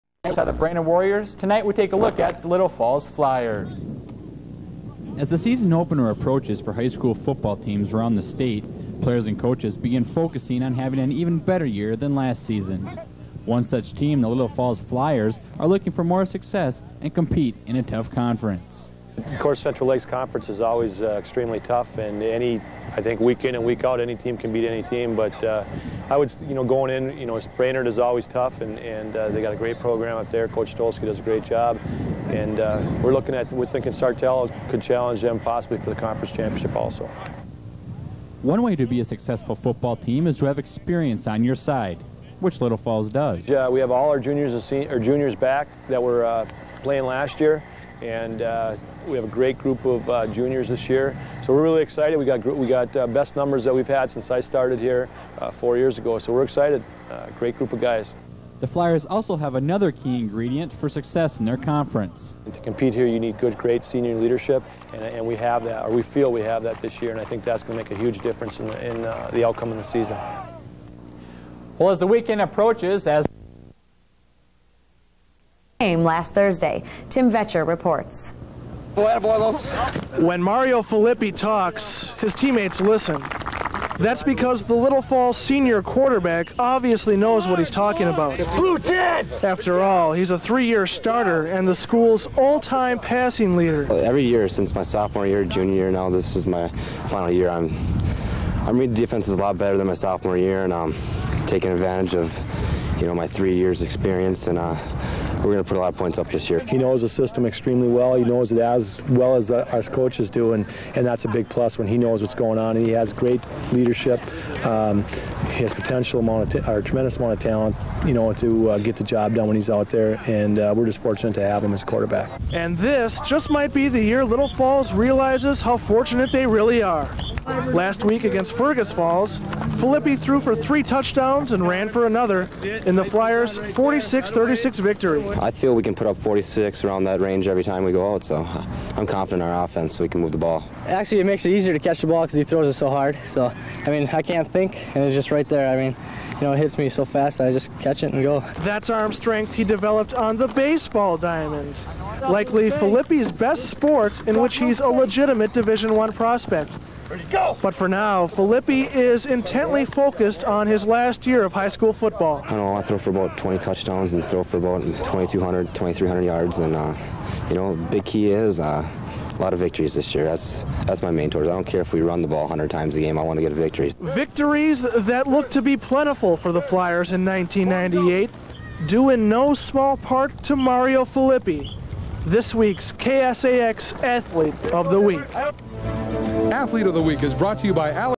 1998 LFCHS Football Mario Fillippi Athlete of the Week Segment
You're watching the 1998 LFCHS Football Mario Fillippi Athlete of the Week Segment from the archives of Flyer Media Productions on the LFCStv® YouTube Channel. Enjoy!